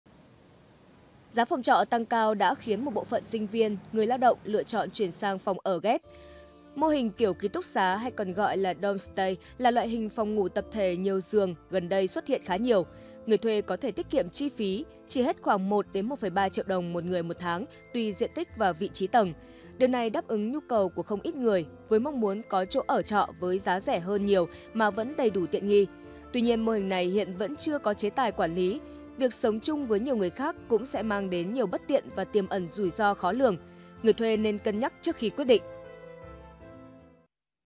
Nở rộ mô hình phòng trọ kiểu ký túc xá | Tin tức
Giá phòng trọ tăng cao đã khiến một bộ phận sinh viên, người lao động lựa chọn chuyển sang phòng ở ghép